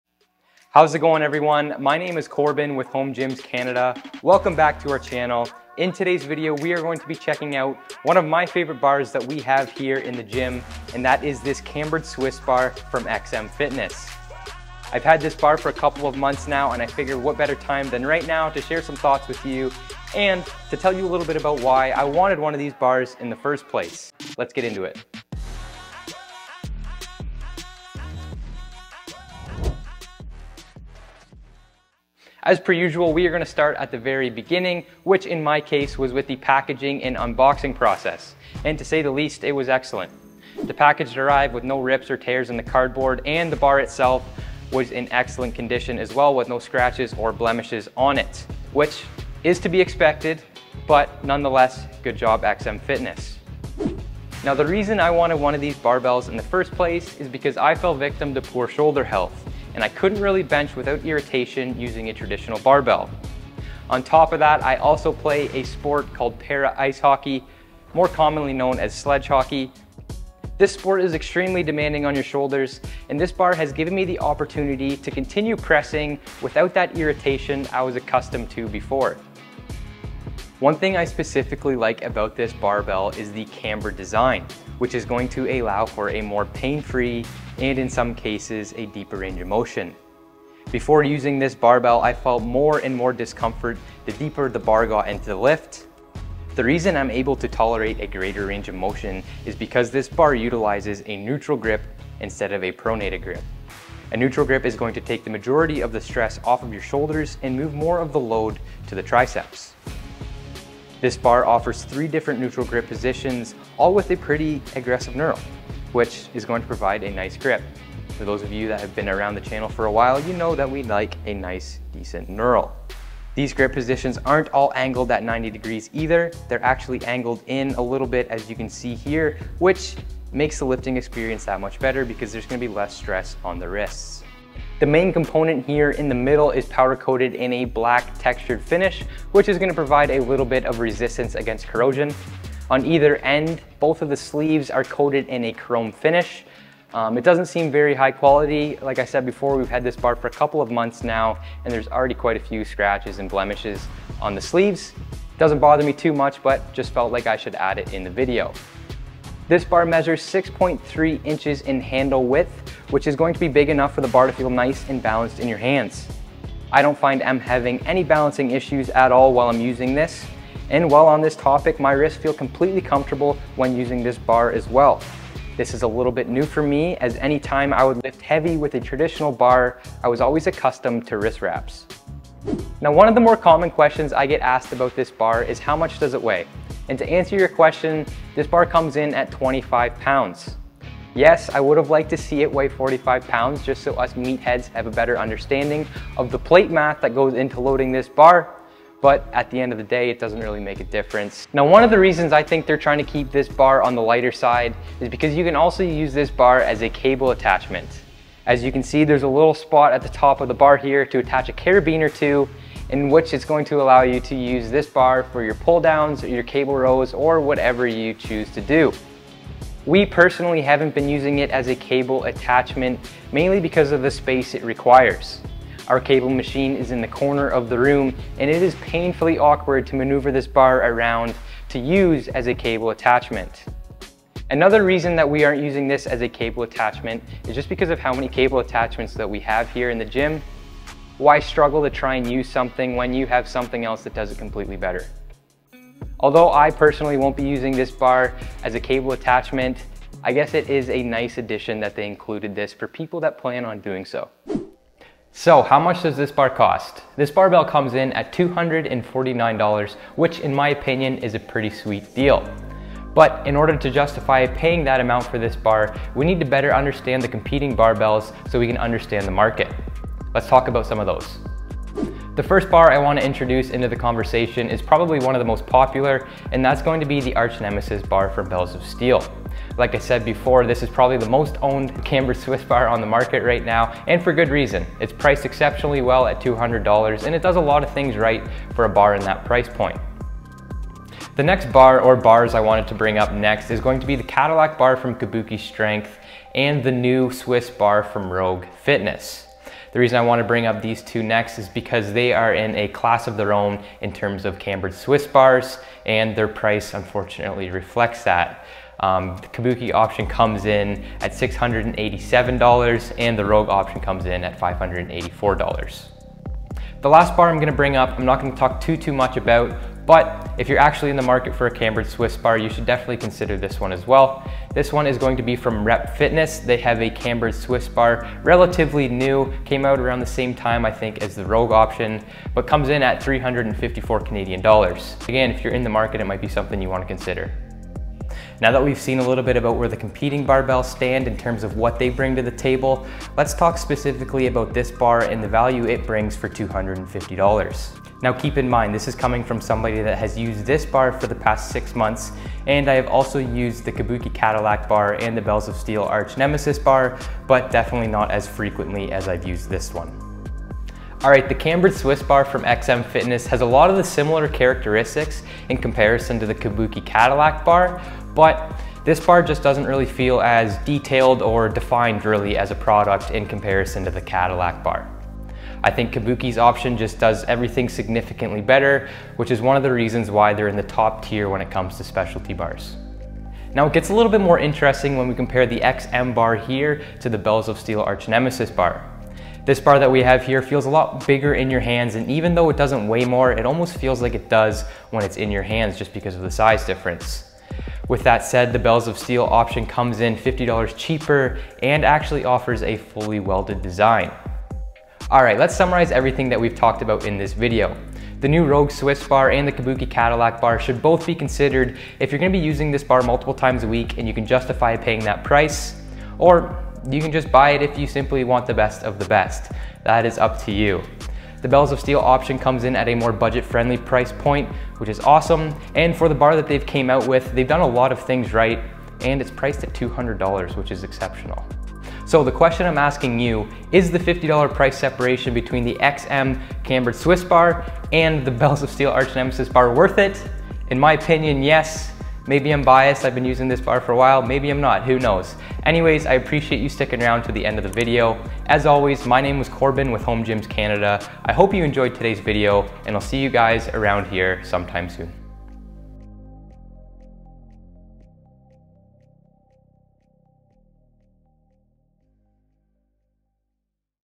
The Reason I ALWAYS Use This Specialty Bar! | XM Cambered Swiss Bar Review!
Hey there! Thanks for tuning into our video, we hope you enjoy(ed)! In today's video we are checking out one of my personal favourite barbells that we own. Not only is it one of my favourites but it's also one of the most used barbells in the gym as well. I originally wanted to pick up one of these barbells because I was experiencing some irritation when pressing with a traditional barbell, specifically during the lowest part of the lift. This irritation was caused from a variety of factors, one being the early years of my lifting journey I was benching at least 3 times every week. This amount of volume, combined with poor programming and sequencing was probably the largest contributing factor.

Following my early years I started a fitness program more tailored to my athletic career and it wasn't until the past 2 - 3 years I started noticing that irritation I was referring to in the video. This bar doesn't directly solve this issue, however it did give me an avenue to continue pressing and in some cases re-establish the lower range of the bench press movement. To this day my shoulder/rotator health has come a long way which is due to proper programming and sequencing as well as lots of rehab/prehab work.

I think this bar has a lot to offer especially if you are experiencing some of the issues I went through a few years ago. As I mentioned earlier, I used to press A LOT, during that time I used various different exercises and methods to excel the progress as fast as possible. I was doing bench press variations like Pin Presses, Resisted or Supported Pressing w/ Bands, Close Grip Pressing, Paused Pressing, etc. I think someone might benefit from this barbell if they are more focused towards powerlifting and are looking to add some variety to their movements. I know this bar would have made my benching journey much more fun and enjoyable if I had it back then.

Lastly, if you have any questions about my shoulder health journey, my current or past lifting programs, sledge hockey/my athletic career, or anything related to home gym equipment, I would love to chat about it with you in the comments below! Thanks again!

Bars Mentioned In This Video!

Home Gyms Canada Links

Timestamps
00:00 - 00:31 Intro
00:32 - 00:53 Unboxing
00:54 - 01:29 Why I Wanted One
01:30 - 03:21 Overview
03:22 04:13 Cable Attachment
04:14 - 04:31 Price
04:32 - 05:37 Comparable Bars
05:38 - 06:44 XM's Value
06:45 - 07:10  Summary
07:11 - 07:43 Outro